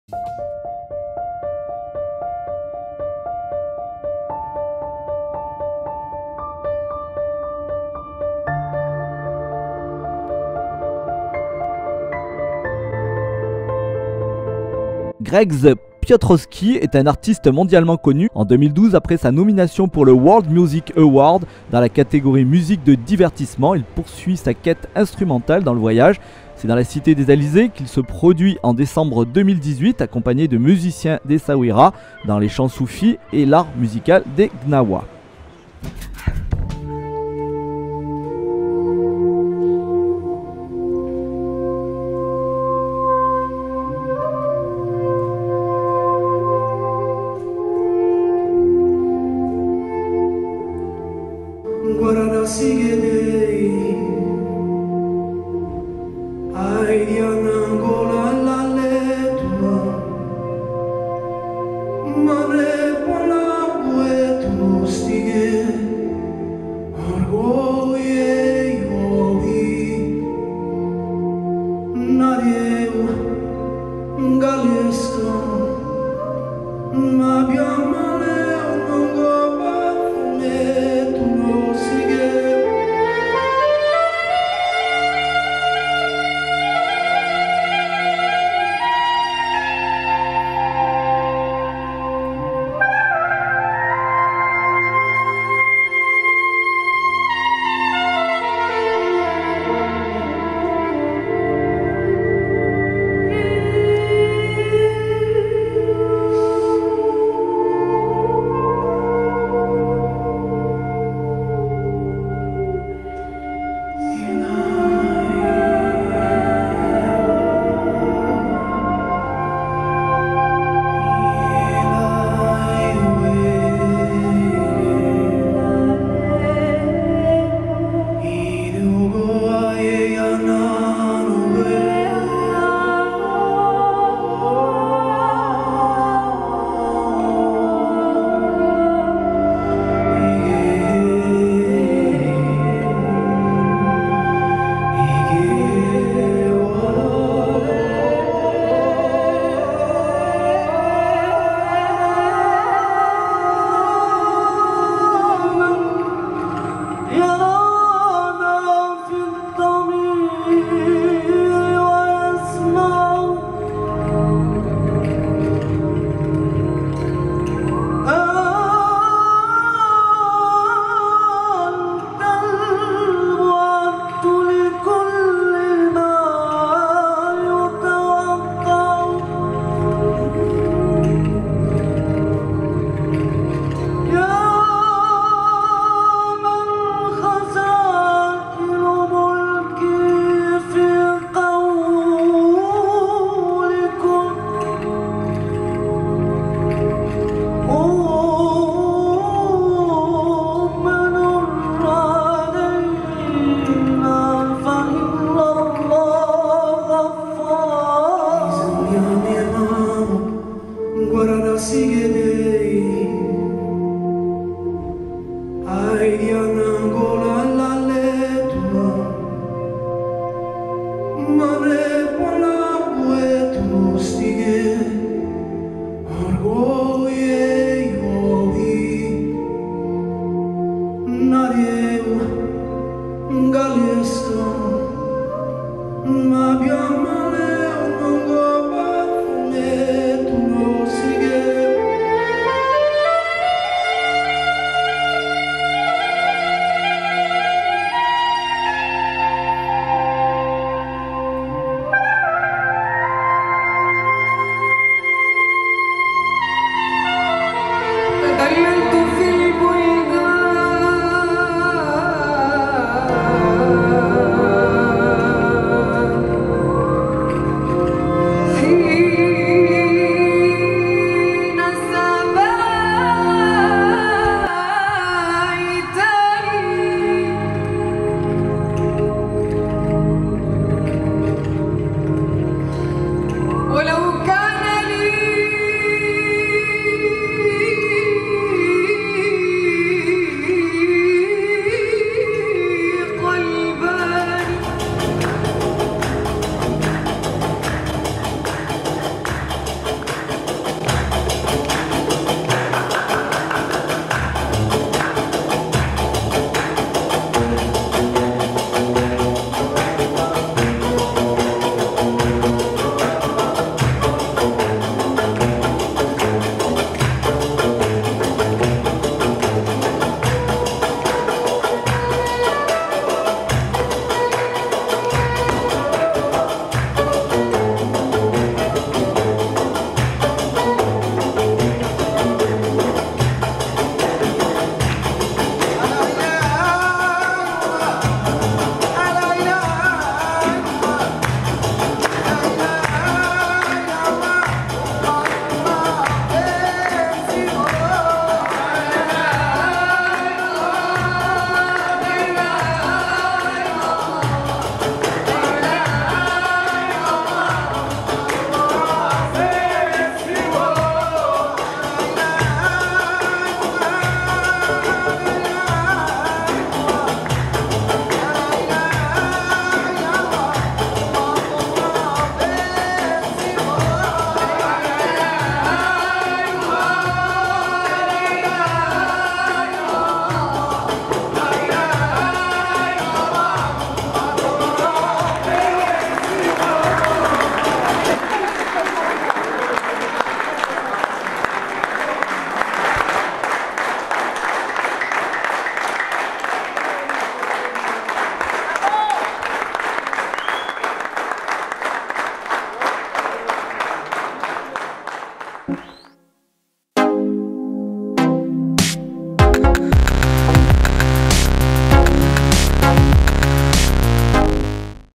Grzech Piotrowski with Morocco musicians (LIVE ESSAOUIRA)
World Music with Grzech Piotrowski and GHOSTMAN, during a "December Captation" with Abdou (vocal) and Samia (vocal). Gnaoua Music in the end of this live.